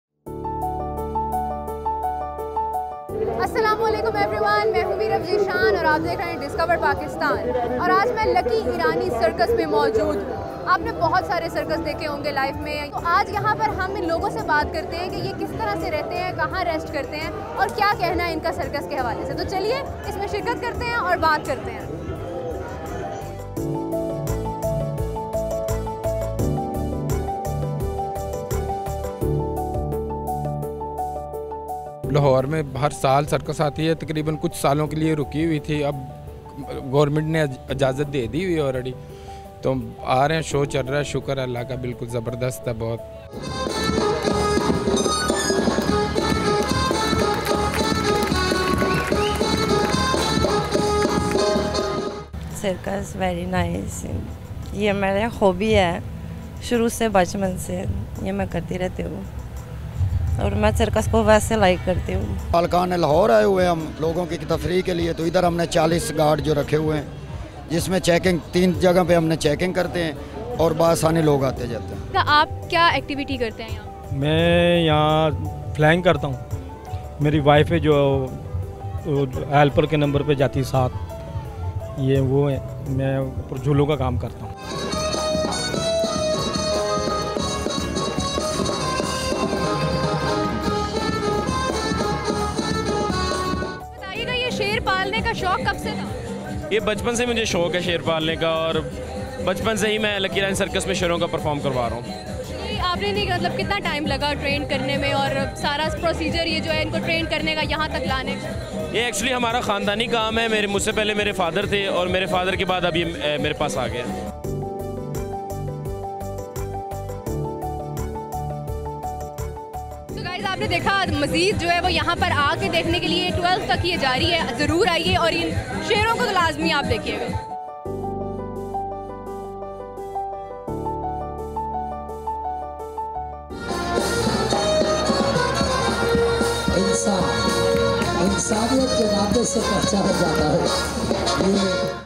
Amazing Lucky Irani Circus | Discover Pakistan TV
In this video, our host visits Lucky Irani Circus and shows us how this circus entertains the people with amazing skills.